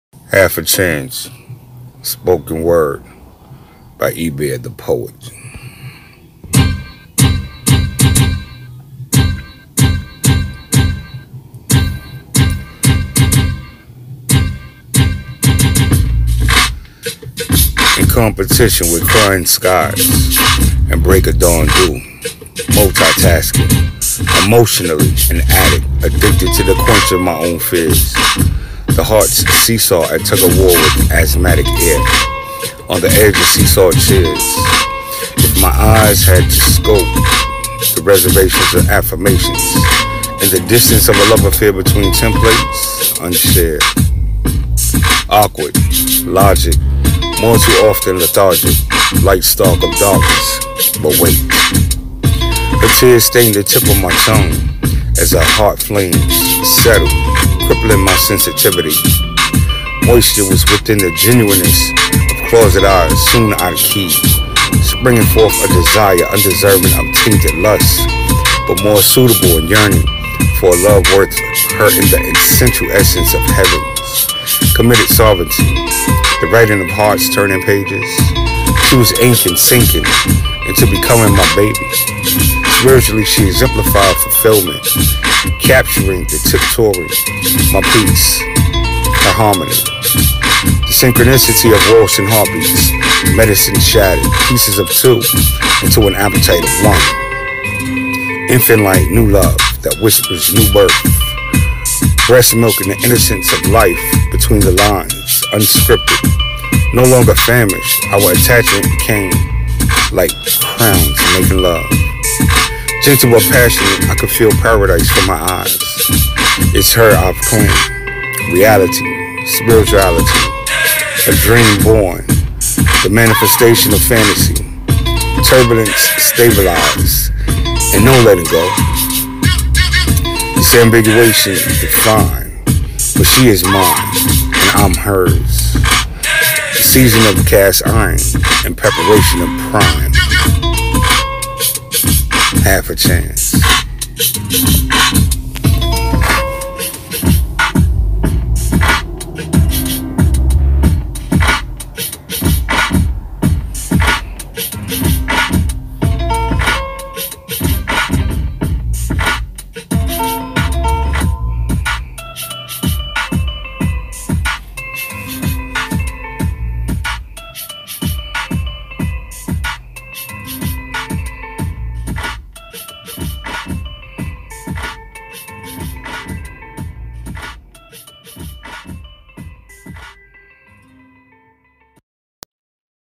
EBear Tha' Poet- "Half A Chance" Spoken Word
Written By Me Eric EBoogie Nunt White
Spoken By Me Eric EBoogie Nunt White
Spoken Word Piece